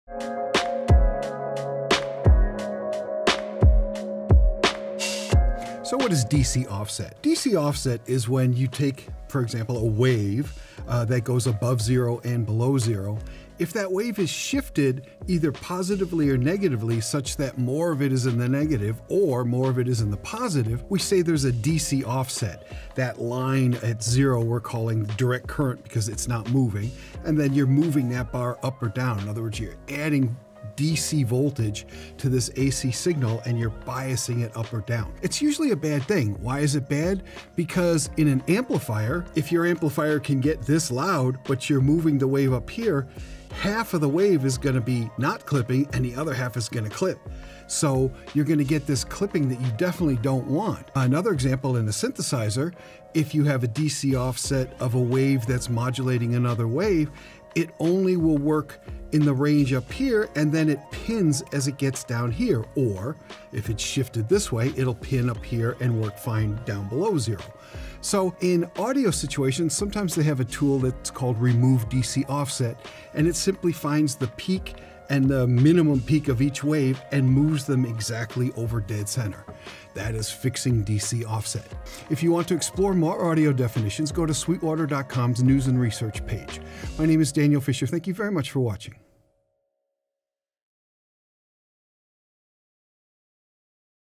What Is DC Offset? – Daniel Fisher
If you’re wondering what video gear we use, check it out

Daniel Fisher, Sweetwater’s synth guru, explains DC Offset, and how it applies to audio signals.

After you watch, check out Sweetwater.com today for all your music instrument and pro audio needs!

0:00 - Intro
0:05 - Defining DC Offset
0:35 - Why Does DC Offset Matter?
1:21 - Thanks for Watching!